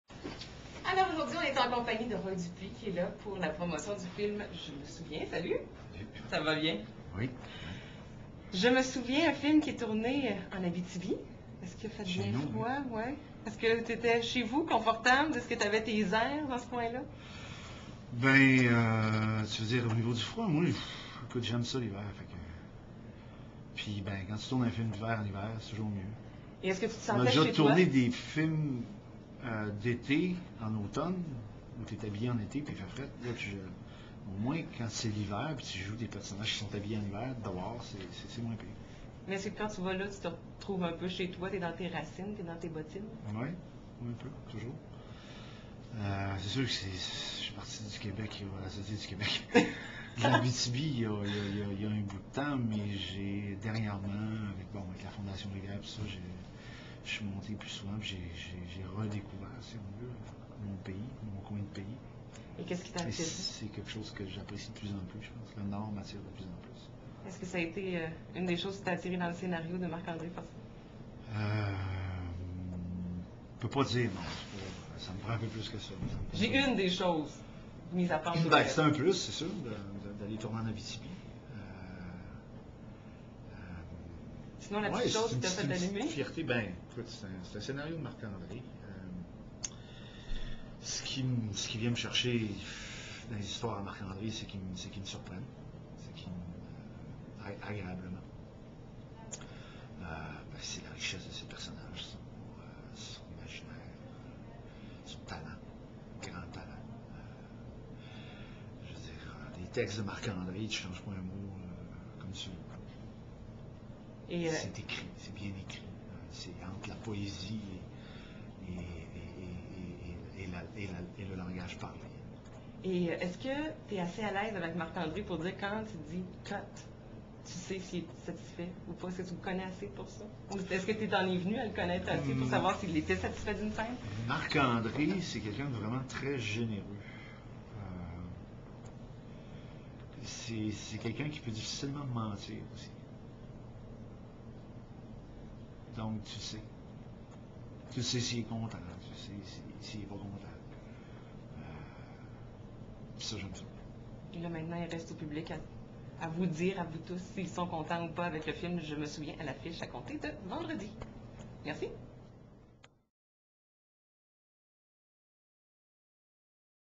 RoyDupuis
Roy Dupuis nous parle du film "Je me souviens" qui prend l'affiche vendredi 6 mars.